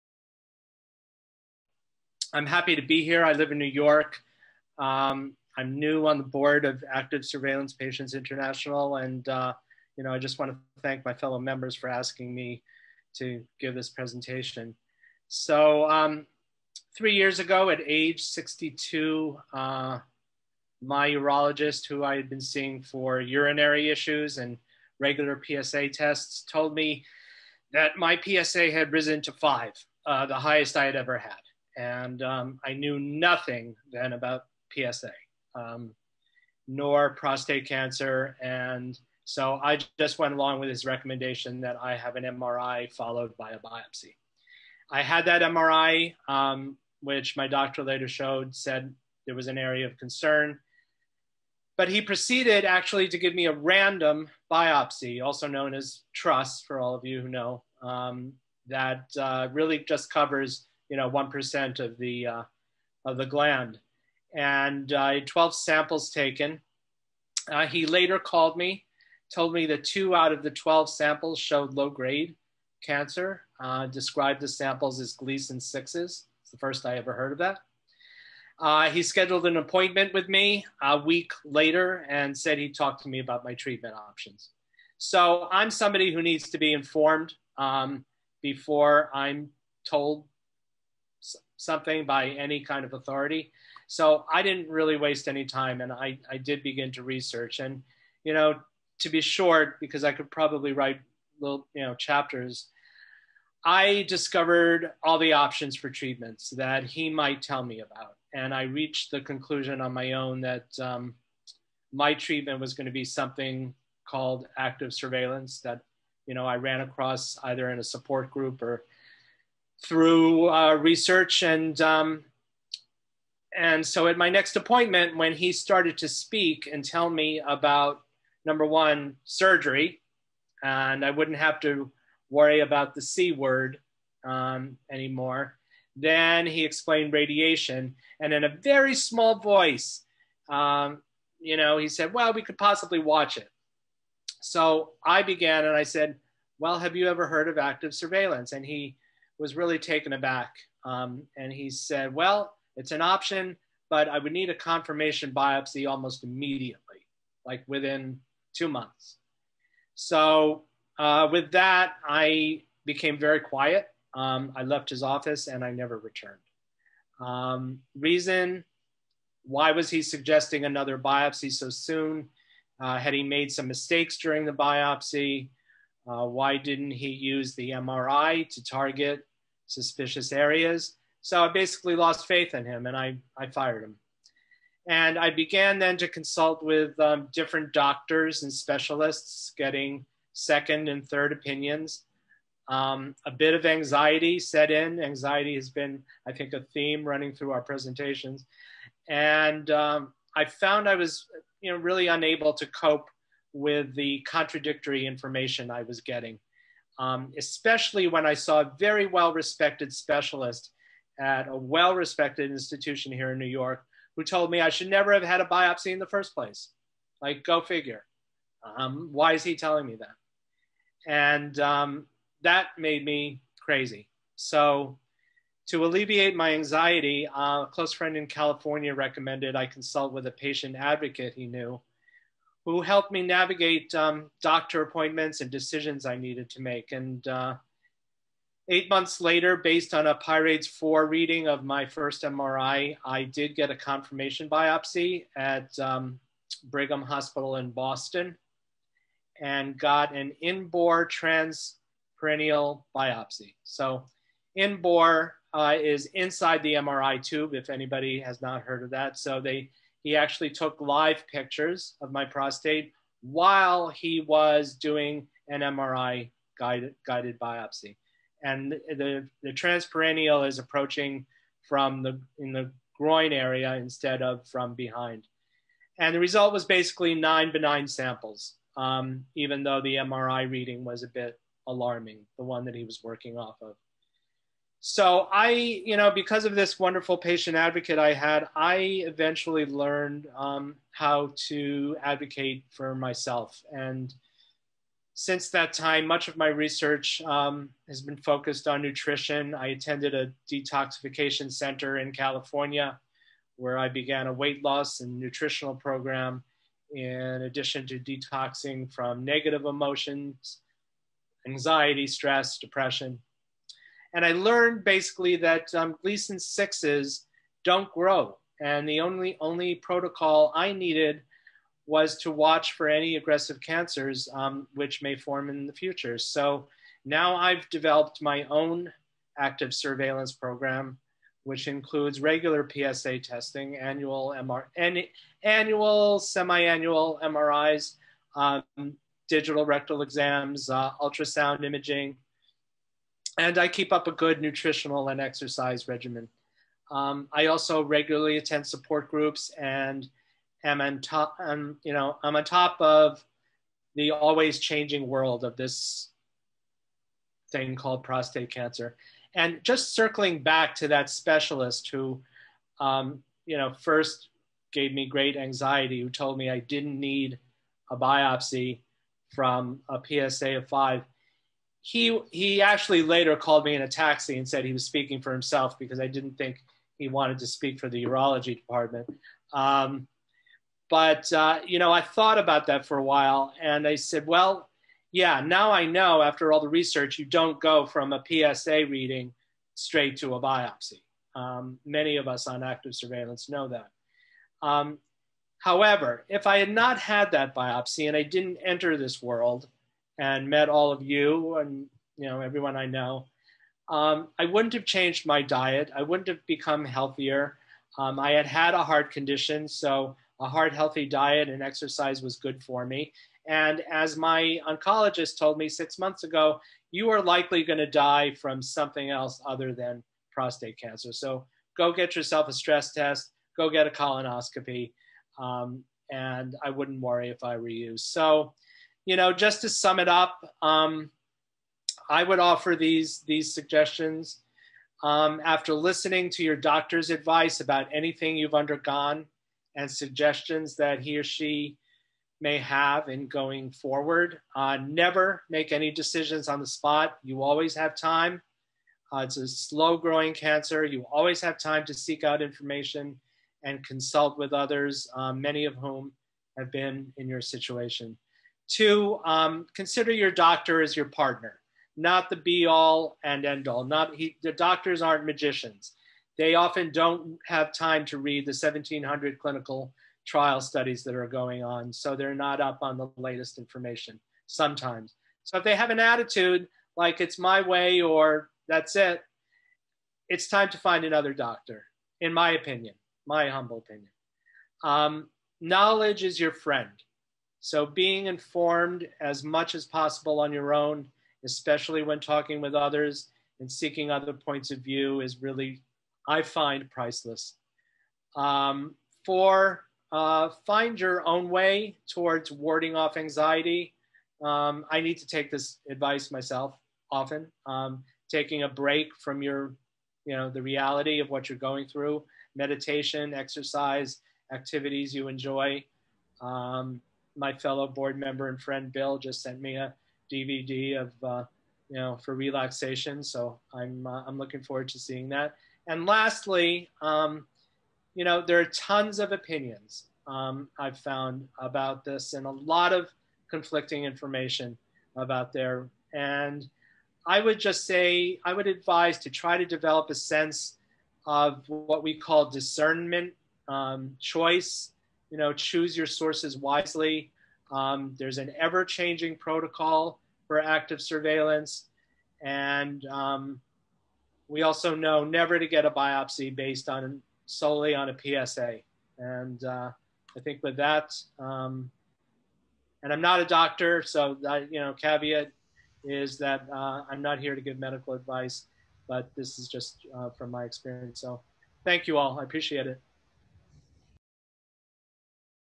ASPI ZOOM JAN 2021 Martin Gerwitz-USA
Active Surveillance Patients International Zoom Meeting January 2021
Martin's personal story